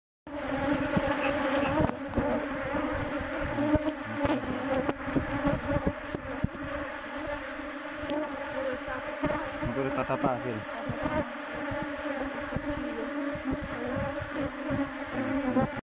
introducción de reinas vírgenes de Apis mellifera
un poco de humo y la reina entra desde la jaula por la piquera.